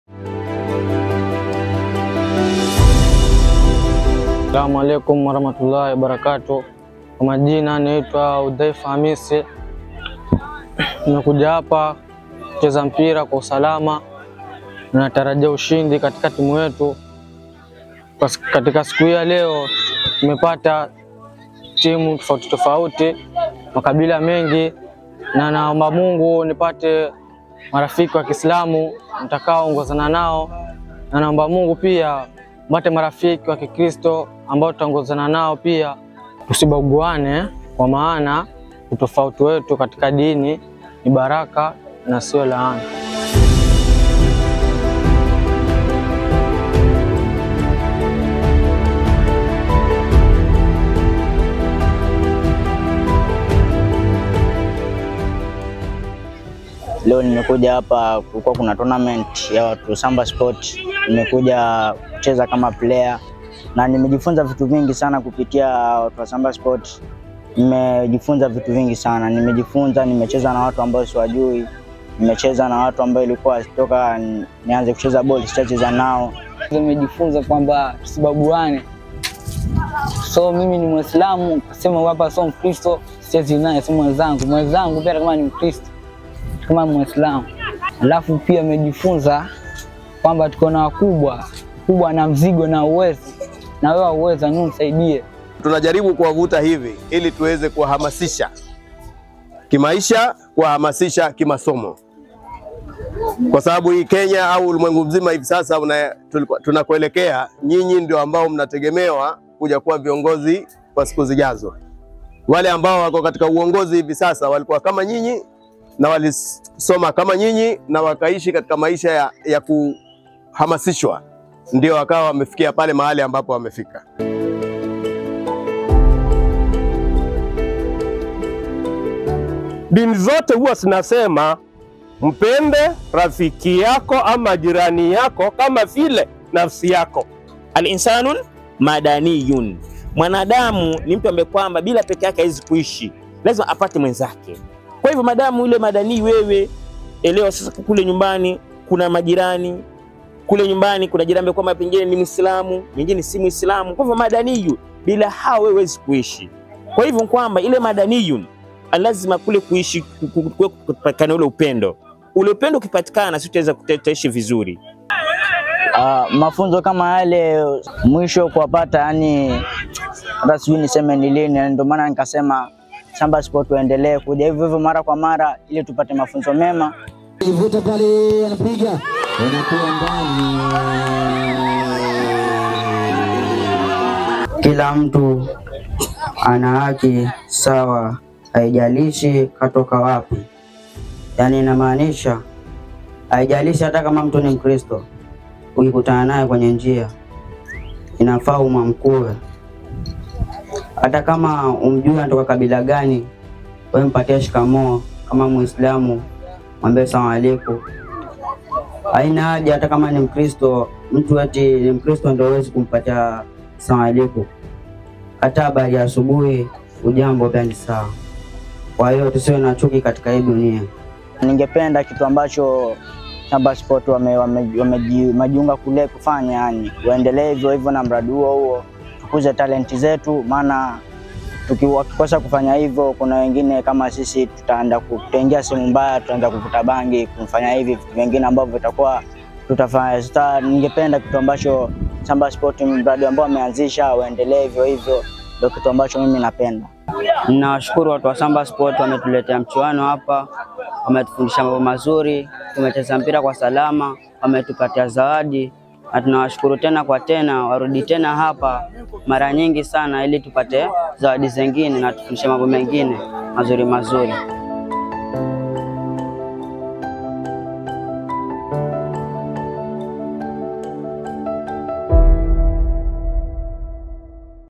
Msambweni Interfaith Soccer 4 Peace tournament.
The tournament brought together adolescents and teenagers from various villages, tribes and religions, to play, have fun, bond, and learn life skills.

This all is in the focus of inspiring cohesion, coexistence and collaboration amongst young people from diverse backgrounds.